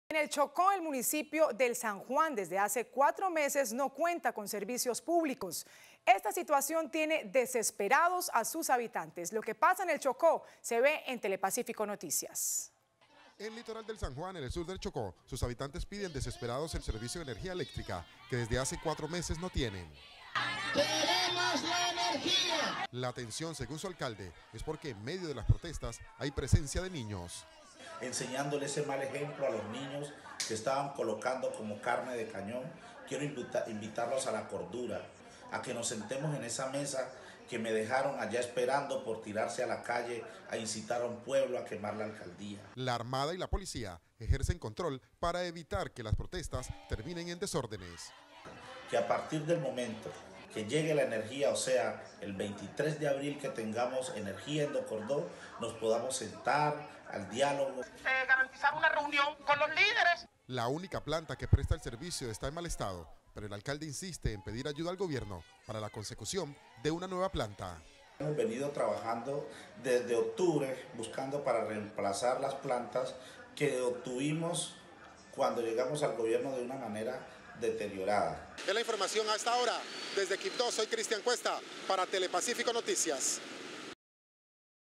Desde hace 4 meses Medio San Juan, Chocó se encuentra sin servicios
Emisión 14 de abril 2018.